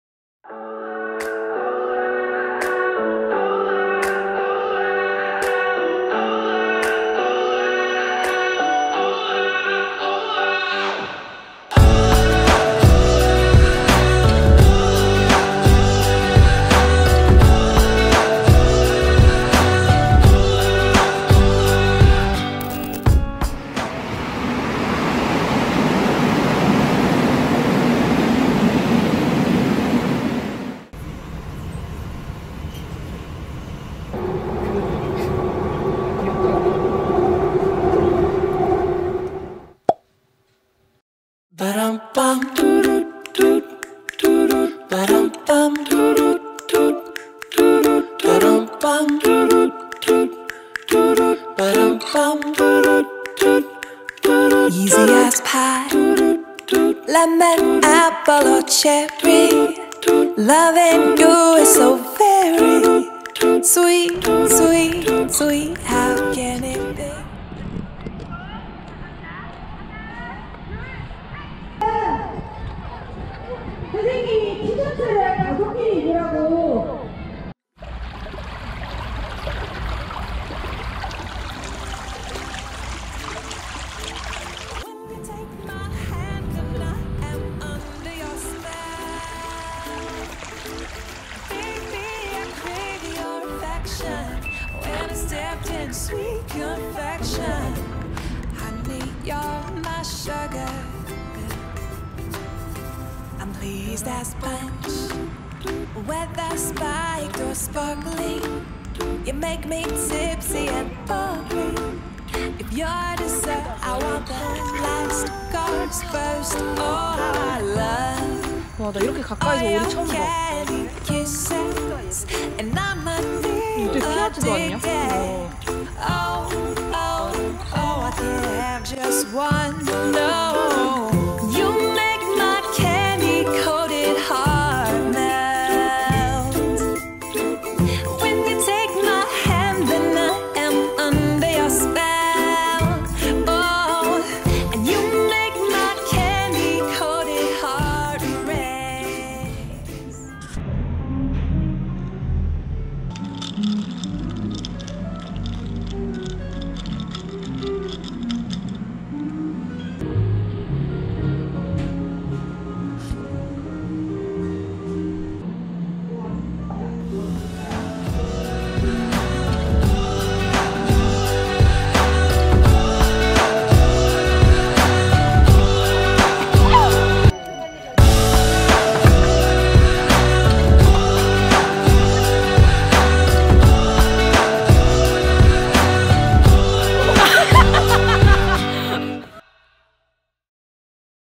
[IFEZ VLOG] 청라 브이로그
10월 청라 브이로그
시원한 바람 솔솔 부는
청라에서 보낸 주말 오후 🍁🍂